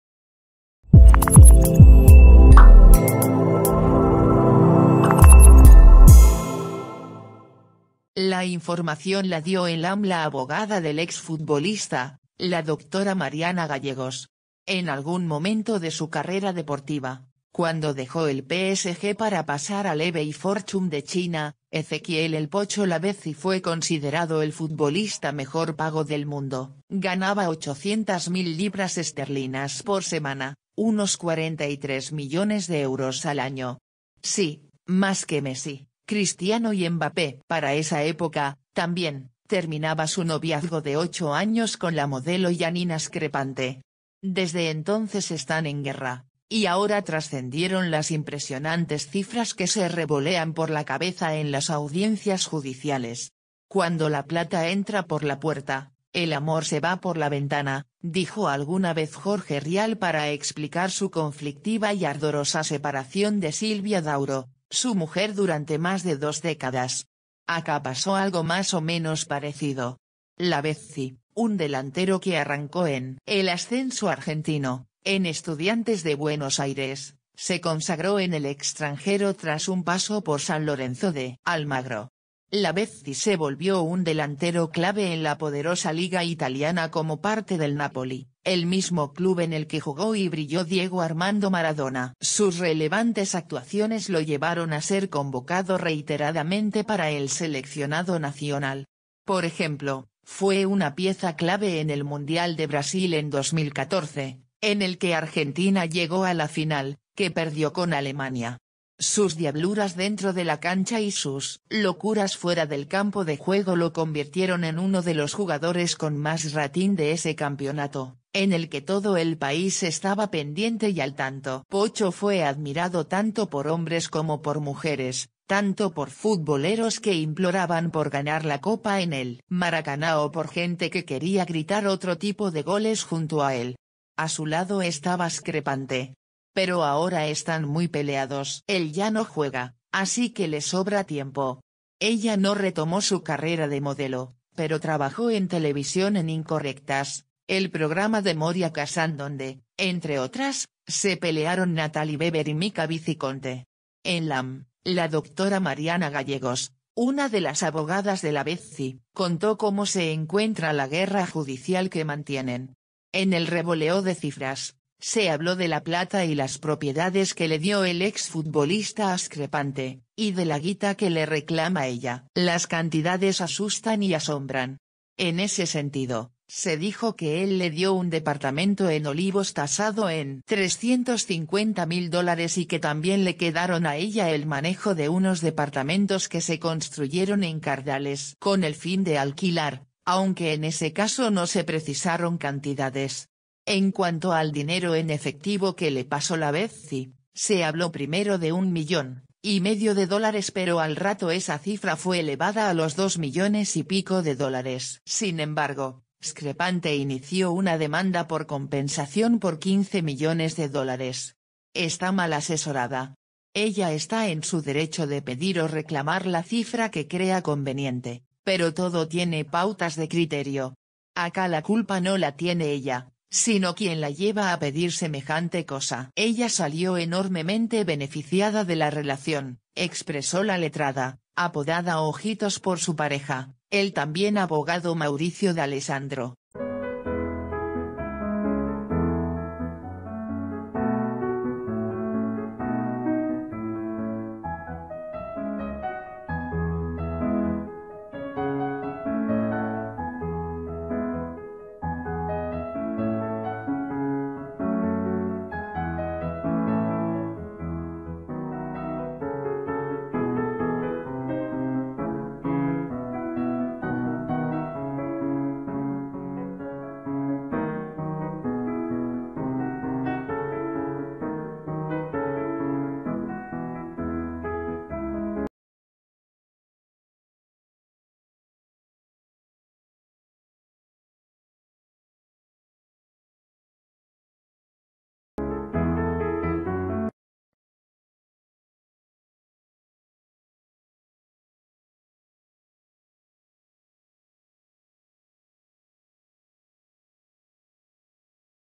¡Guerra de millones! Revelan la fortuna que Pocho Lavezzi le dejó a Yanina Screpante, y la tone...
¡Guerra de millones! Revelan la fortuna que Pocho Lavezzi le dejó a Yanina Screpante, y la tonelada de plata que le reclama ella - Últimas Noticias teleshow

La información la dio en LAM la abogada del ex futbolista, la doctora Mariana Gallegos. En algún momento de su carrera deportiva, cuand...